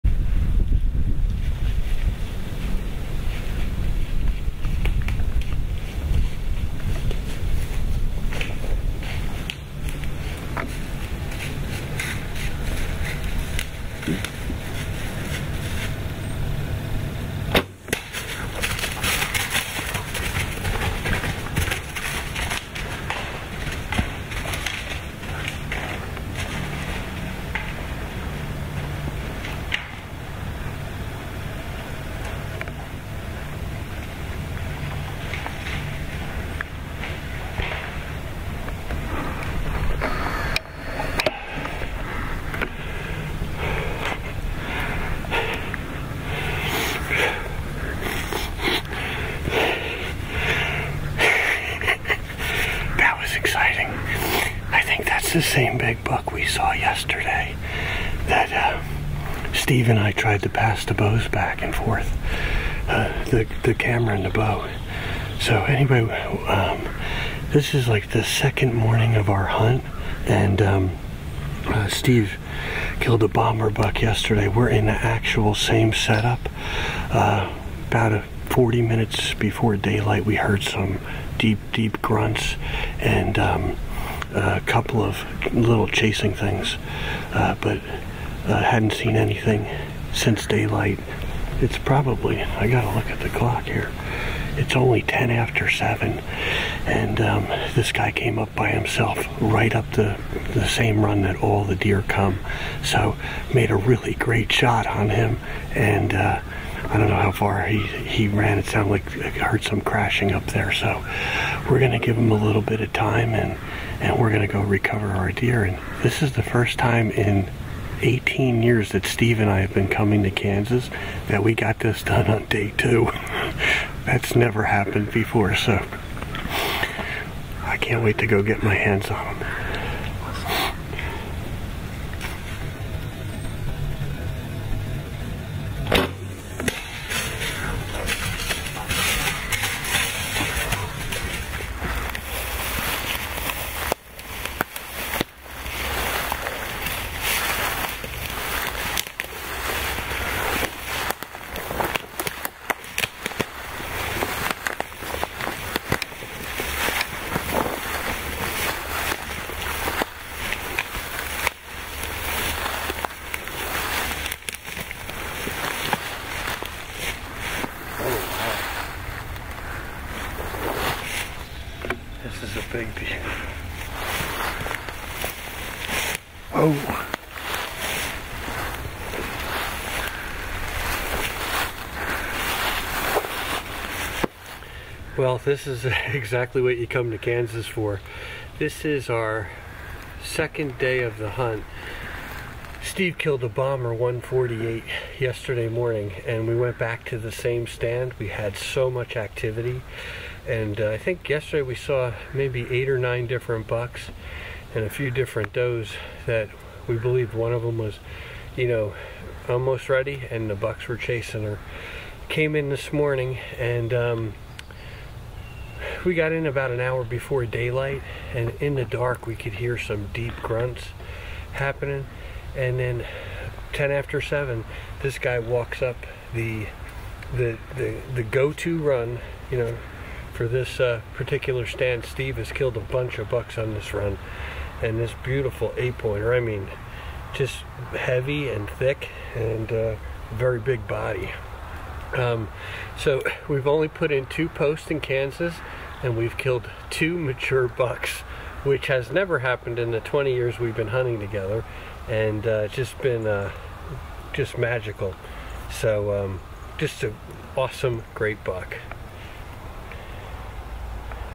Gettin Stickbow Close, Kansas 2020 Part 2! 2 Bucks 2 Days Same Stand, Black Widow Bows, Recurve
Day 2 of our 2020 Kansas Hunt and the 1st deerthat shows up is a Shooter! 2 Bucks @ Days Same Stand!